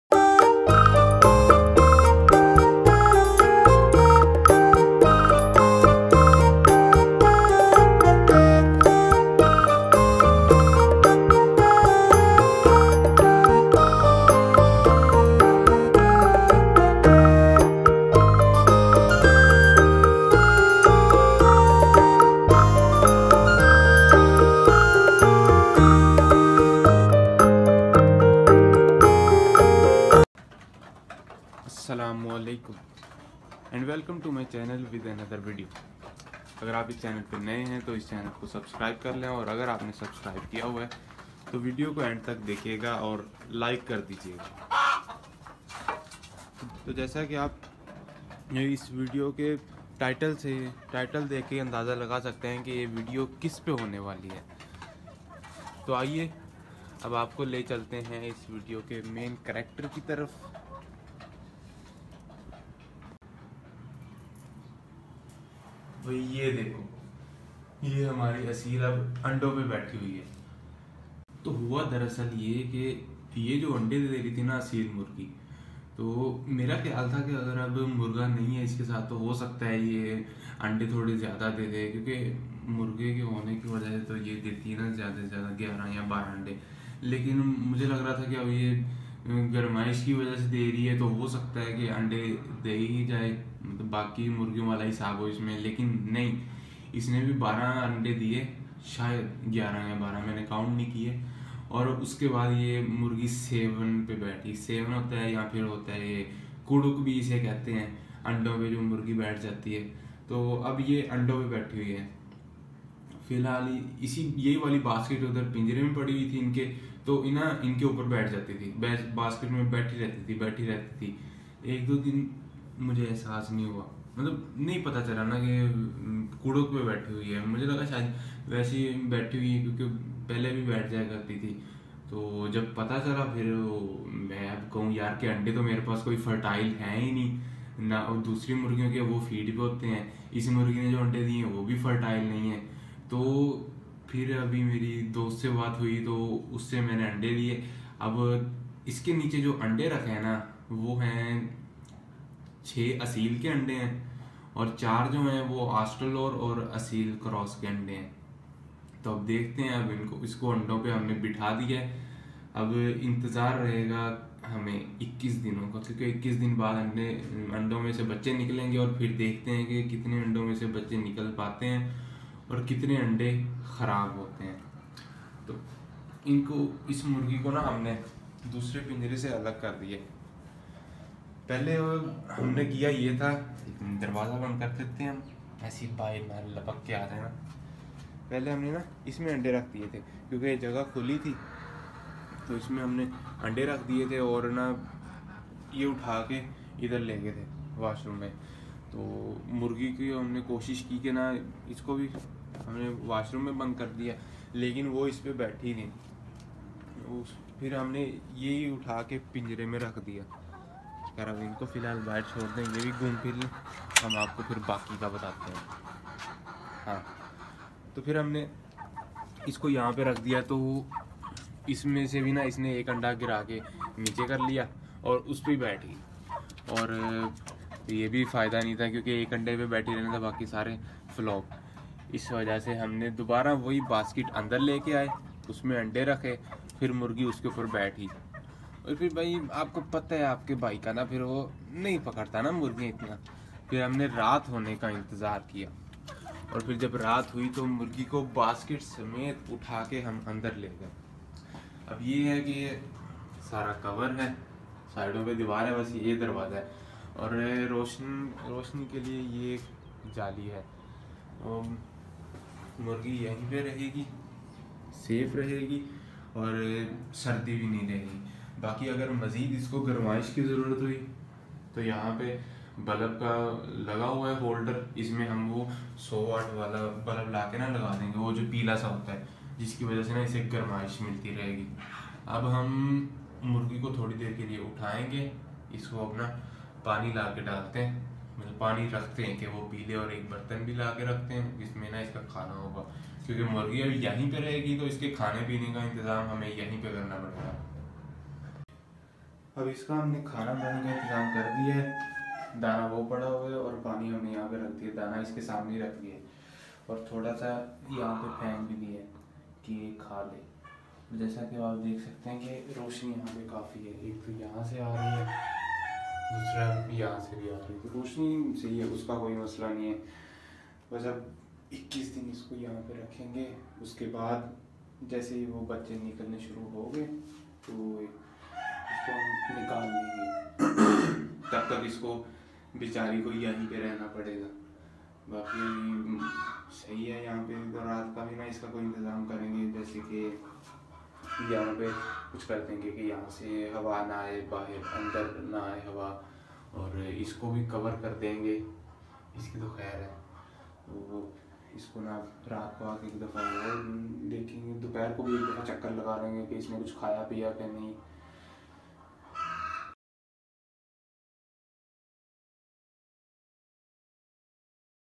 Black Aseel Hen Harvesting Eggs to Chicks | Natural Hatching
In This Video I will show you an aseel hen whose egg hatching process is started